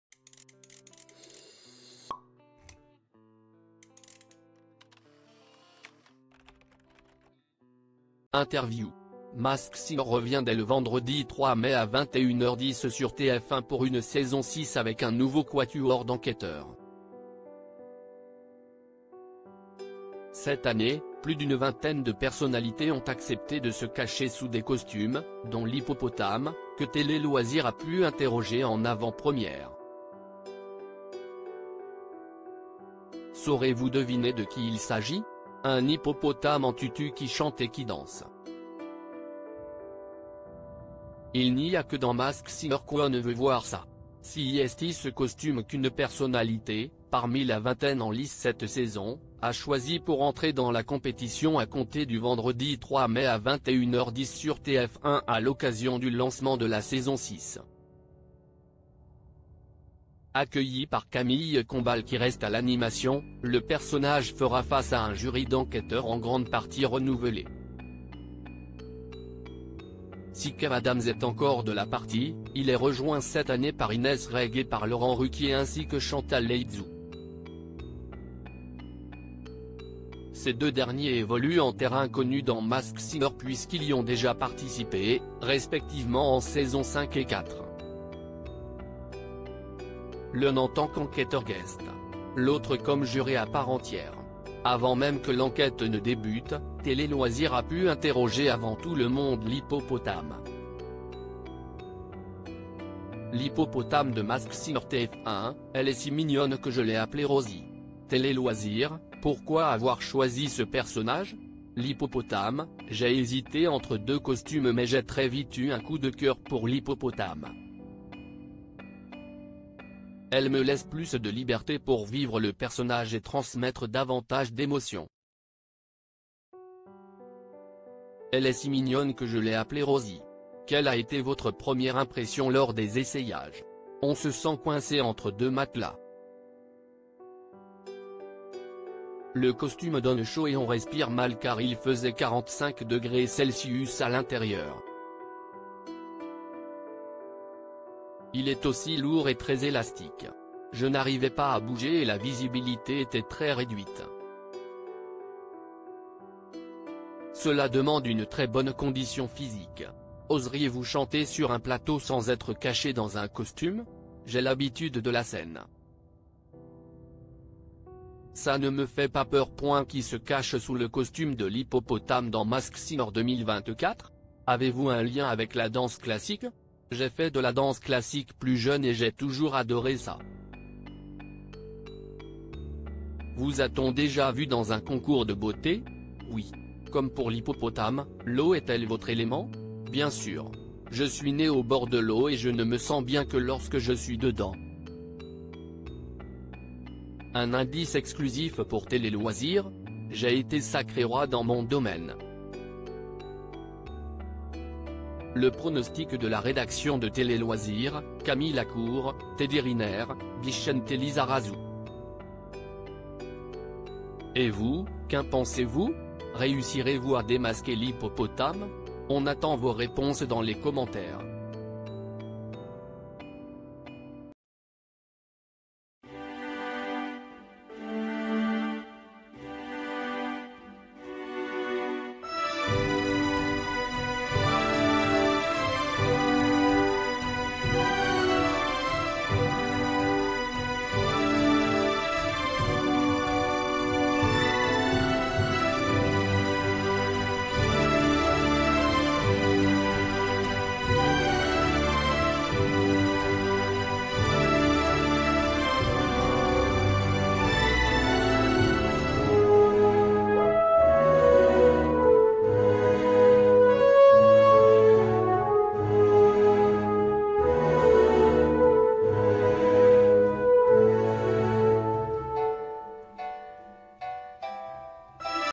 "J'ai l'habitude de la scène" : Qui se cache sous le costume de l'Hippopotame dans Mask Singer 2024
"J'ai l'habitude de la scène" : Qui se cache sous le costume de l'Hippopotame dans Mask Singer 2024 ? Nos pronostics